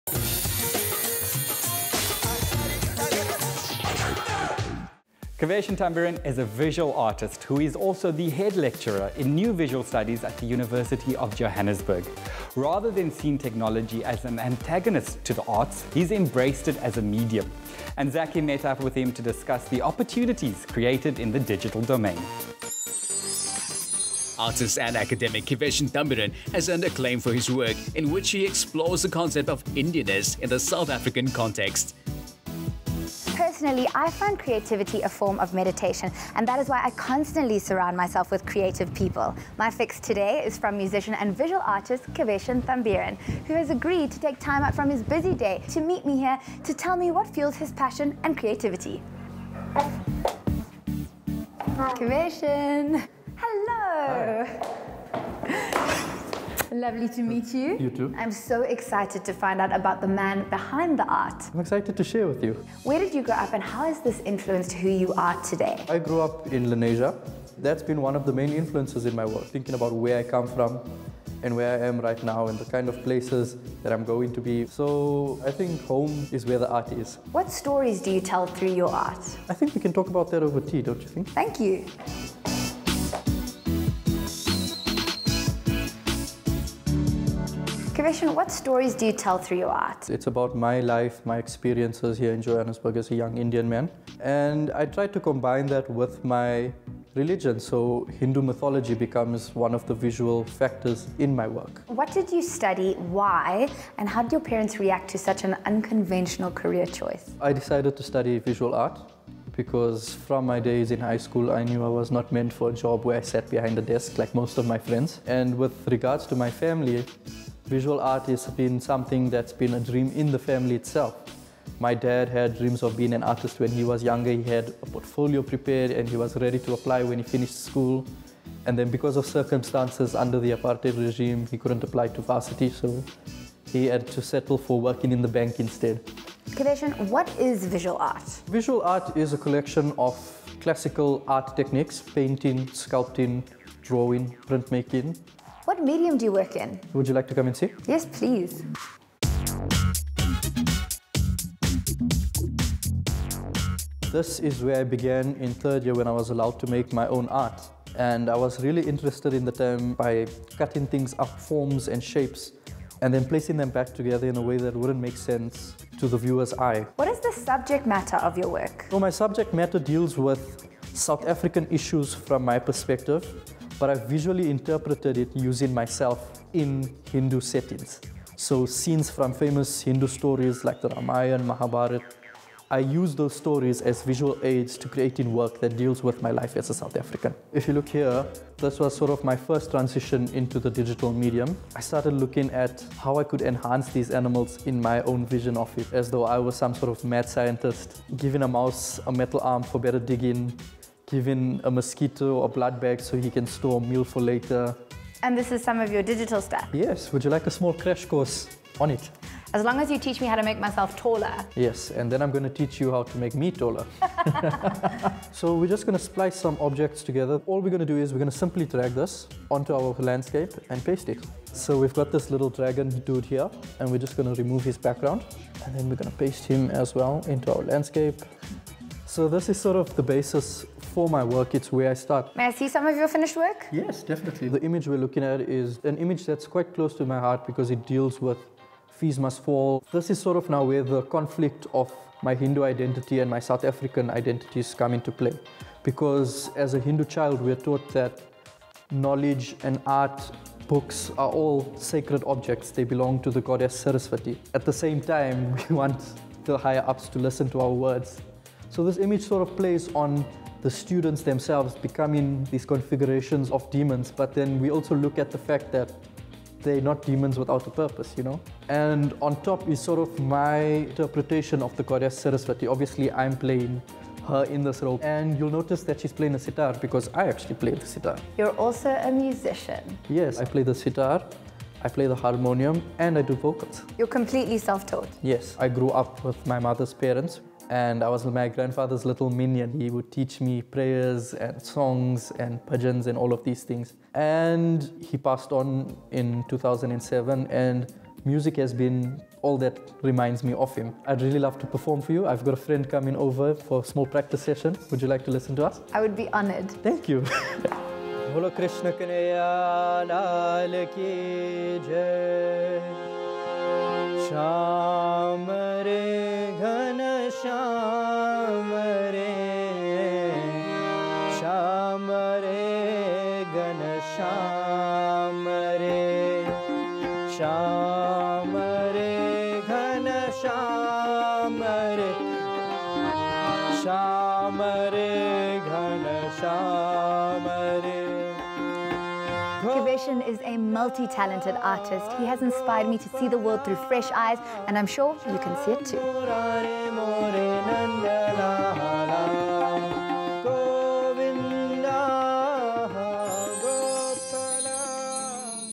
Kiveshan Thumbiran : Artist
Kiveshan Thumbiran is a visual artist and instead of seeing technology as an antagonist to the arts, he’s embraced it as a medium.